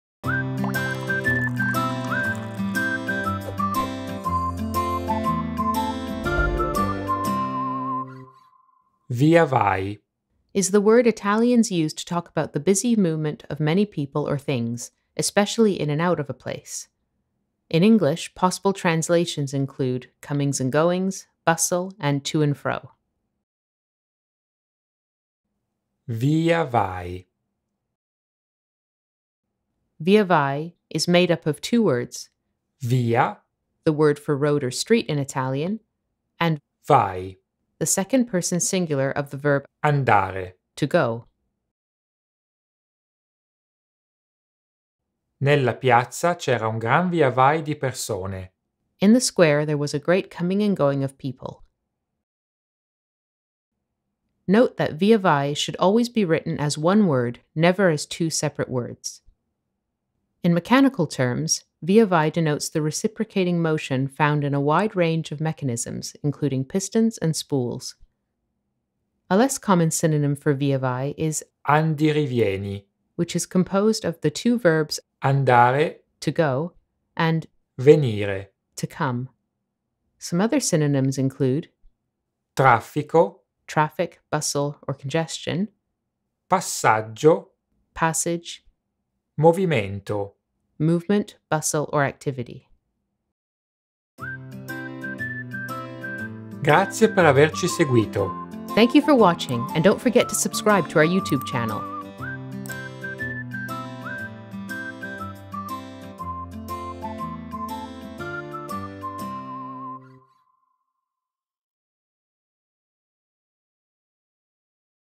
How to Say "Comings and Goings" in Italian - Italian Word of the Day
The Italian word of the day is “Viavai” (Comings and goings). Learn more about this word and how to use it in our latest video.